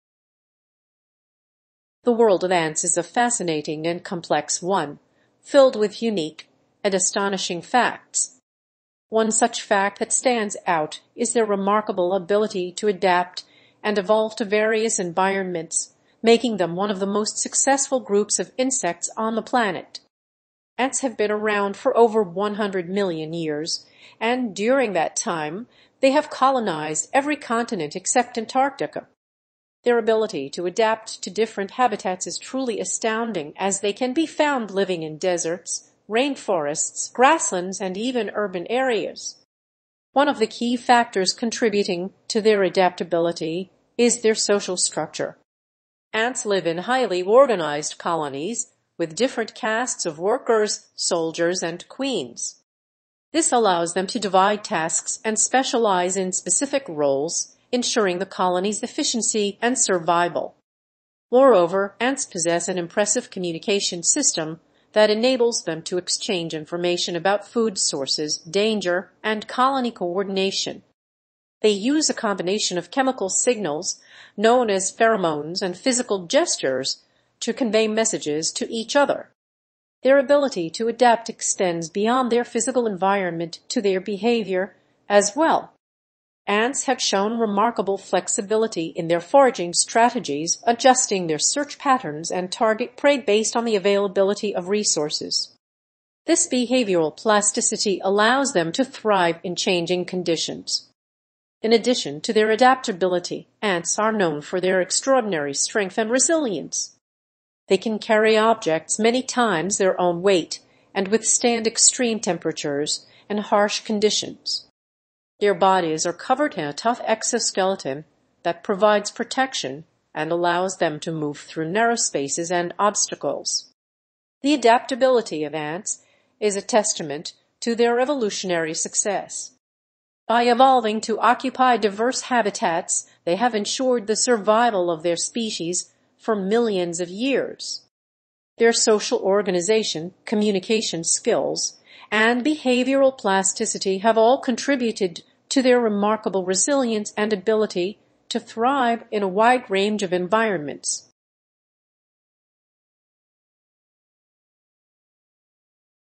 0000114 - The Ever-Evolving World of Ants
The world of ants is a fascinating and complex one, filled with unique and astonishing facts. One such fact that stands out is their remarkable ability to adapt and evolve to various environments, making them one of the most successful groups of insects on the planet.

Ants have been around for over 100 million years, and during that time, they have colonized every continent except Antarctica. Their ability to adapt to different habitats is truly astounding, as they can be found living in deserts, rainforests, grasslands, and even urban areas.

One of the key factors contributing to their adaptability is their social structure. Ants live in highly organized colonies, with different castes of workers, soldiers, and queens. This allows them to divide tasks and specialize in specific roles, ensuring the colony's efficiency and survival.

Moreover, ants possess an impressive communication system that enables them to exchange information about food sources, danger, and colony coordination. They use a combination of chemical signals, known as pheromones, and physical gestures to convey messages to each other.

Their ability to adapt extends beyond their physical environment to their behavior as well. Ants have shown remarkable flexibility in their foraging strategies, adjusting their search patterns and target prey based on the availability of resources. This behavioral plasticity allows them to thrive in changing conditions.

In addition to their adaptability, ants are known for their extraordinary strength and resilience. They can carry objects many times their own weight and withstand extreme temperatures and harsh conditions. Their bodies are covered in a tough exoskeleton that provides protection and allows them to move through narrow spaces and obstacles.

The adaptability of ants is a testament to their evolutionary success. By evolving to occupy diverse habitats, they have ensured the survival of their species for millions of years. Their social organization, communication skills, and behavioral plasticity have all contributed to their remarkable resilience and ability to thrive in a wide range of environments.